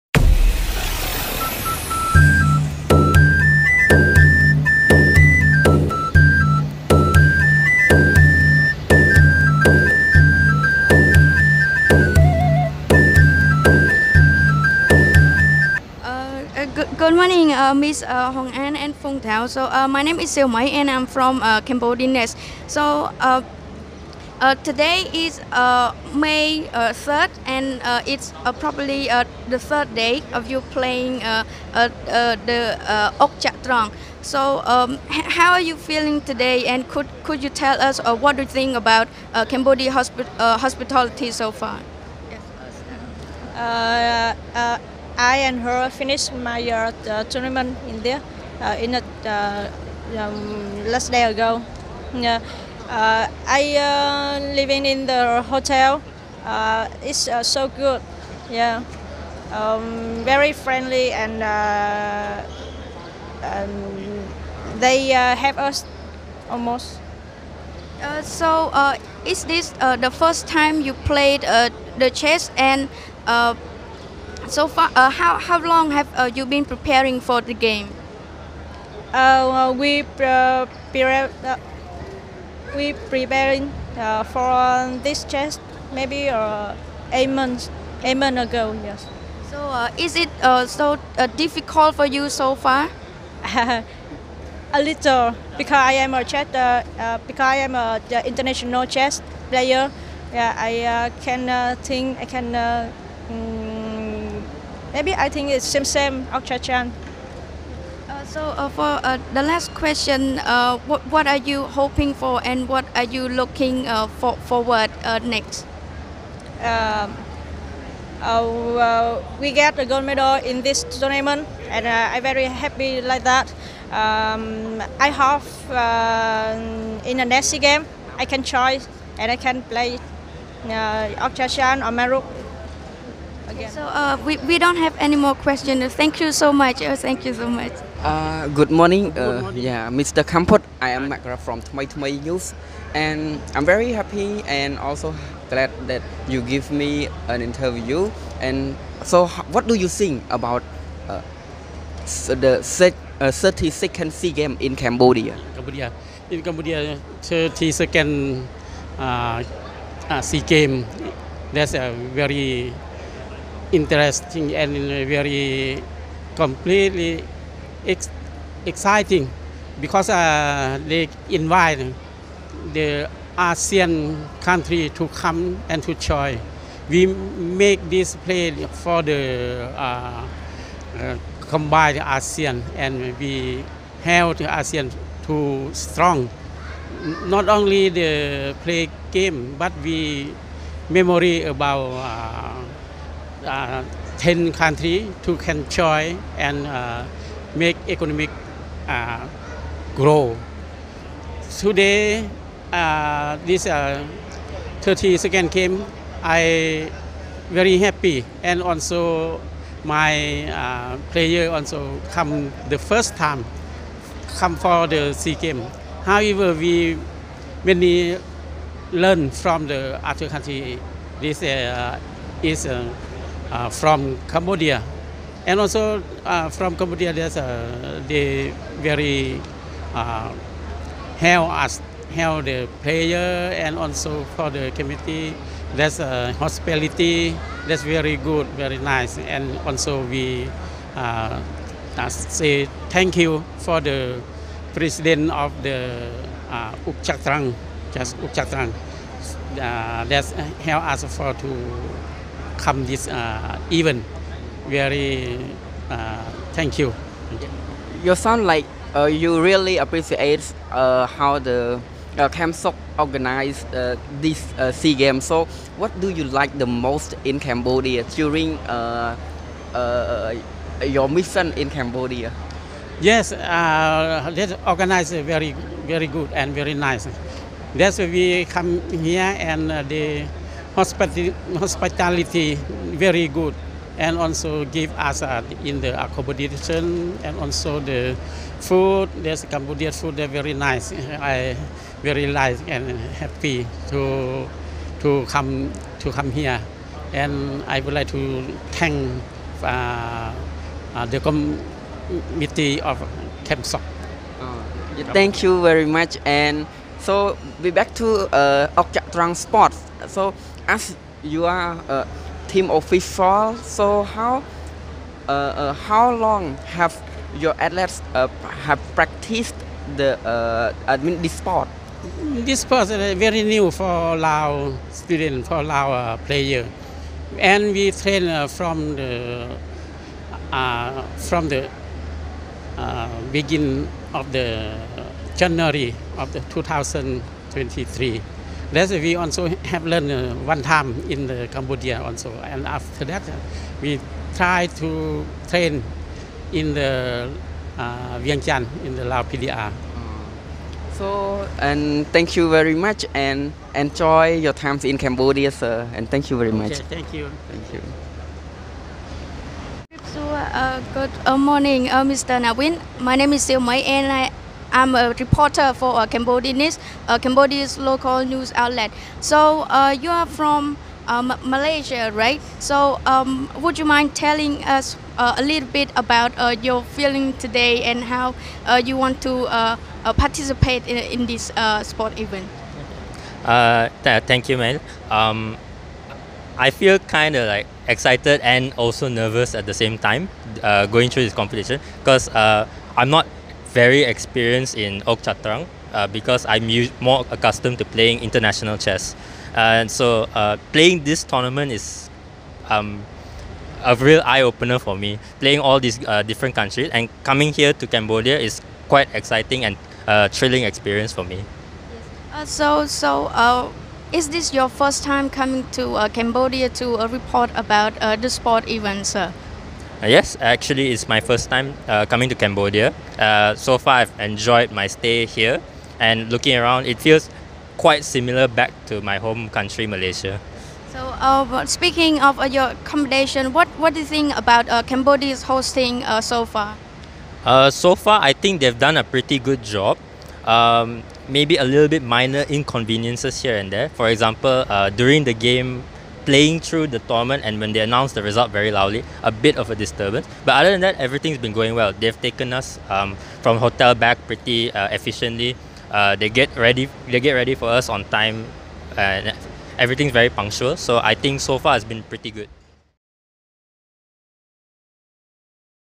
SEA Games: Ouk Chaktrang Delegations Praise Cambodia for Good Hospitality
SEA Games 2023: Ouk Chaktrang ASEAN Delegations Praise Cambodia for Good Hospitality During the 32nd SEA Games. The competition is joined by seven countries: Cambodia, Laos, Malaysia, Myanmar, Philippines, Thailand, and Vietnam.